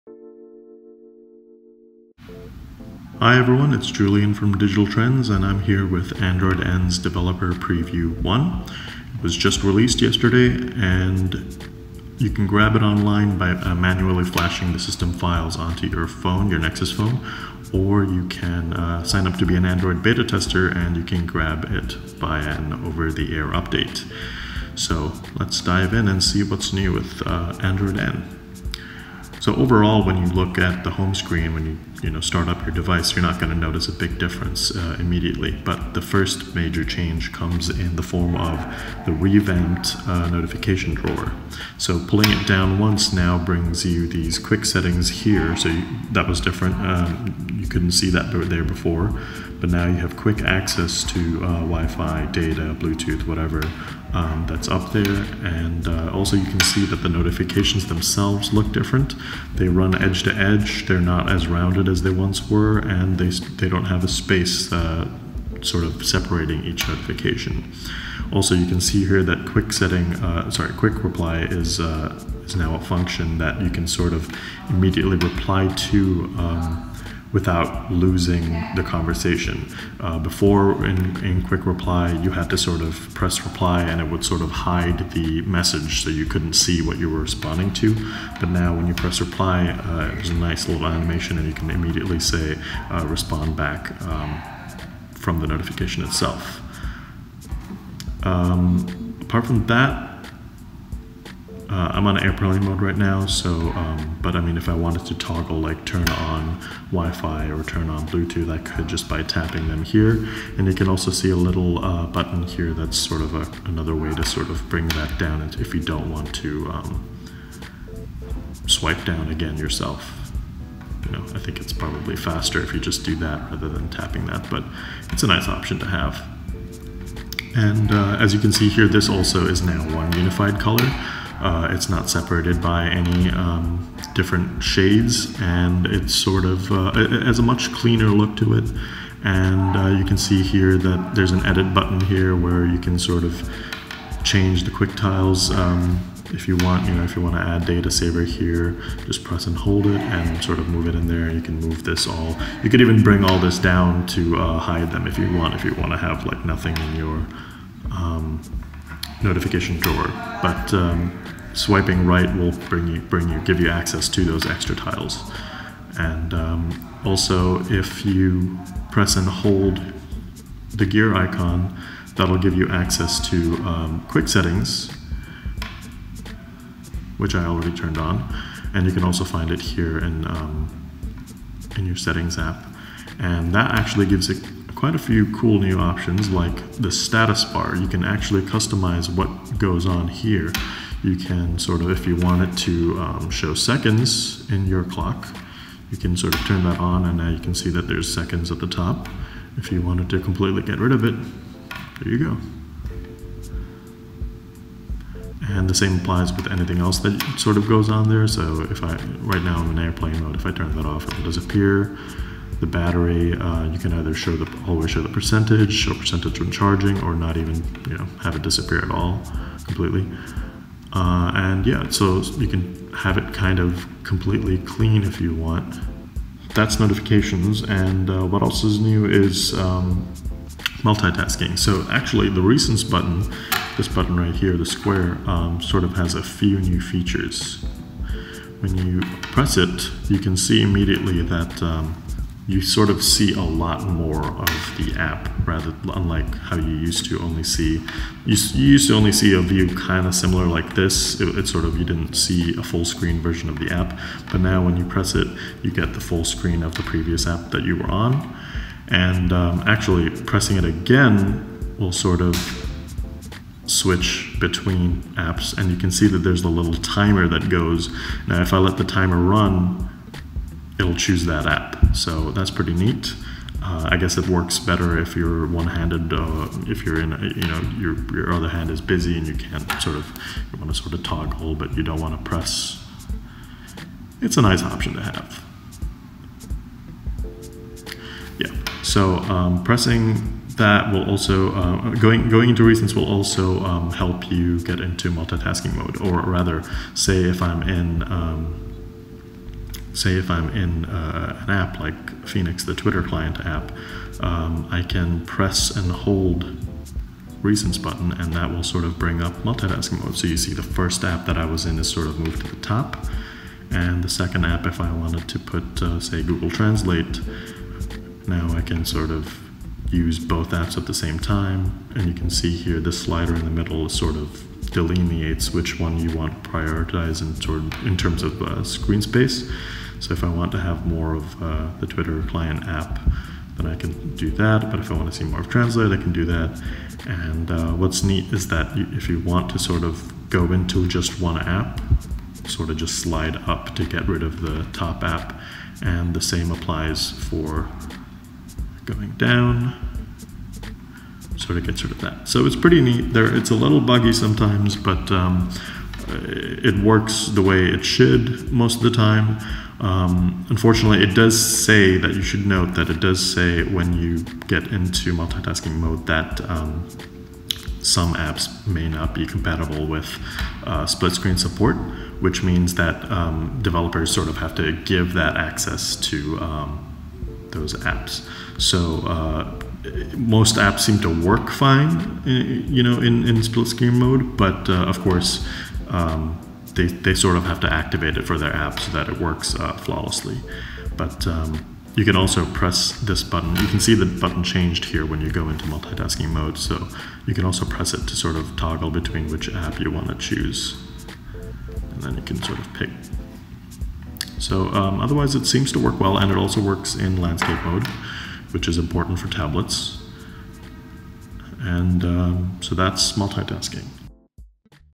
Android N Developer Preview - hands-on
What will the next flavor of Android bring to the party?  We take a look at the first Developer Preview for Android N, with its split-screen functionality and revamped notification drawer.